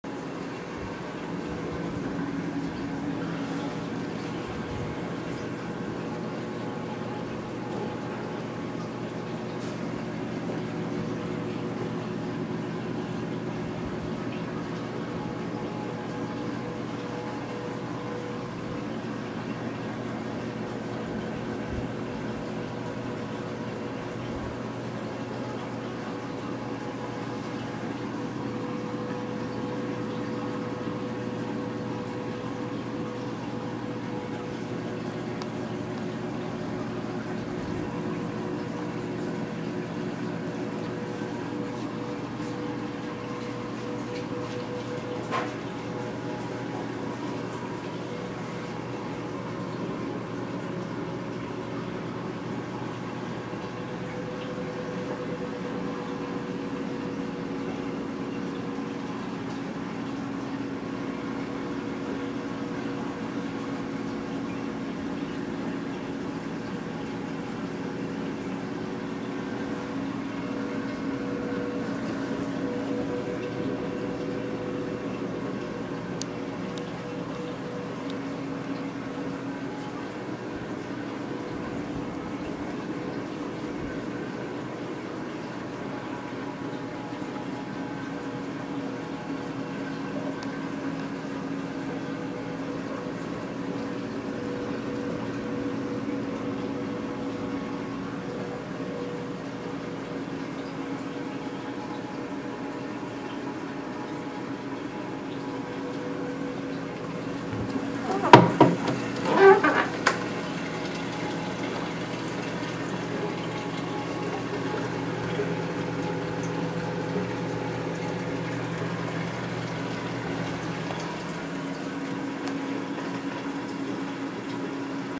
135 gallon reef aquarium
after a few months of everything settling from the transfer from the 55 gallon.